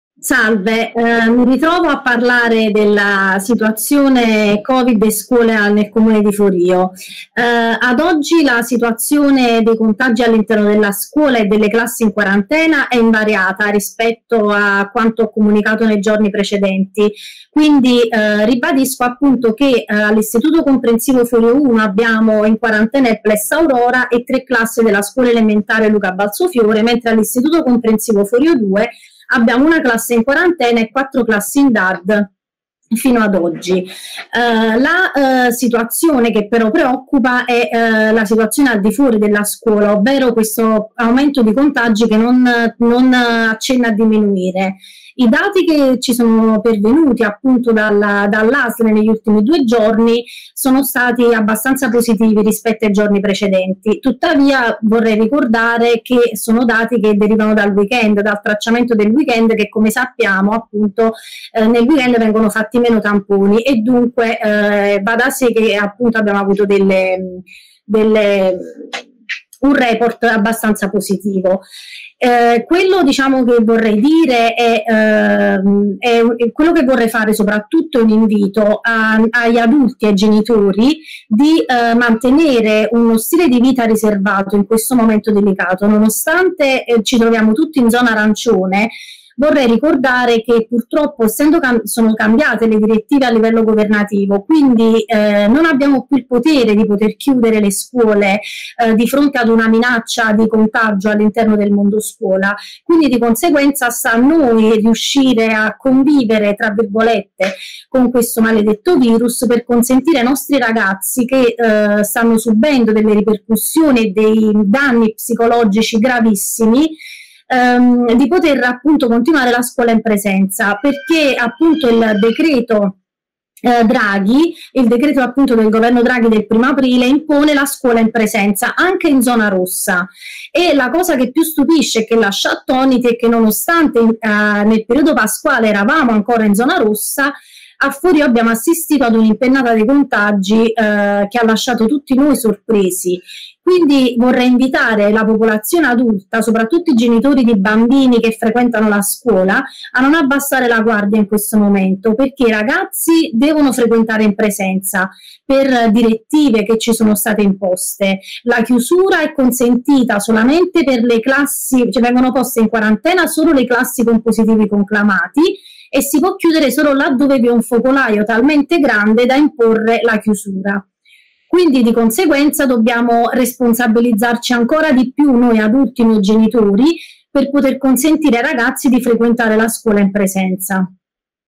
A FORIO SITUAZIONE SCUOLA STABILE. MANUELA ARTURO INVITA TUTTI AD ESSERE RESPONSABILI.
L’aumento vertiginoso dei casi positivi sull’isola d’Ischia nei giorni successivi alla Pasqua, ha creato una situazione preoccupante soprattutto a Ischia e Forio. Nel comune all’ombra del Torrione la situazione al momento è stabile, anche per quanto concerne le classi messe in quarantena presso gli Istituti comprensivi presenti sul territorio. Manuela Arturo, Consigliere comunale con delega all’istruzione, chiede a tutti di tenere un comportamento responsabile per arginare la diffusione del coronavirus.